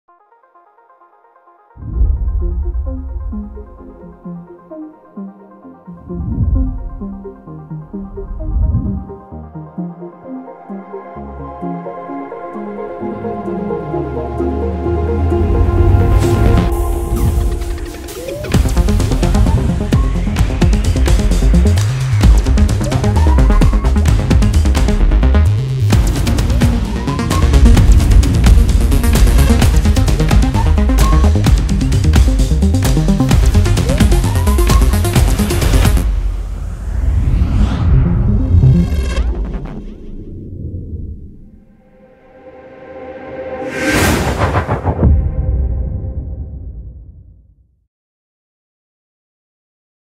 Taycan Light Painting: Porsche Light Teaser
A trailer for the final Porsche Taycan Behind the scenes Light Painting Video.